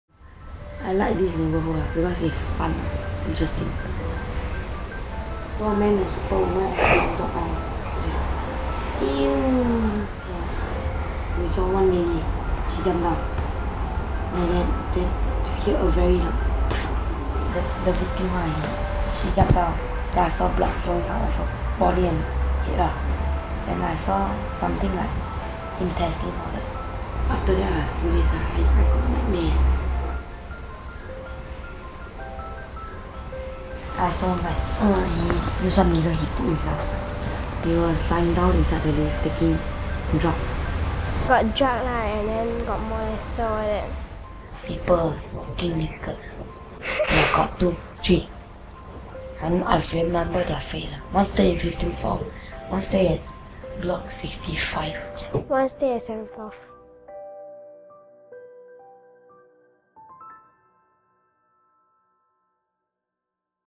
Through their eyes
A look at the complicated neighbourhood surrounding the rental estate in Circuit Road through the eyes of its young residents could be more morbid than many expect. Siblings Gavin and Germaine talk about the suicide and other things they have witnessed around their block.